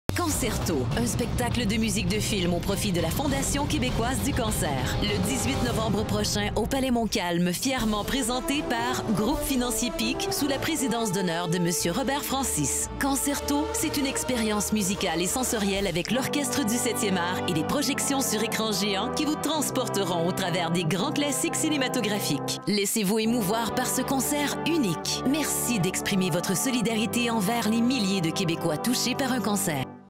Soirée-bénéfice Cancerto Québec 2022
Composé de 50 artistes, l’Orchestre du Septième Art (OSA) vous transportera dans une expérience musicale et sensorielle hors du commun, à la découverte de grandes œuvres cinématographiques. Émotions garanties le 18 novembre 2022 au Palais Montcalm.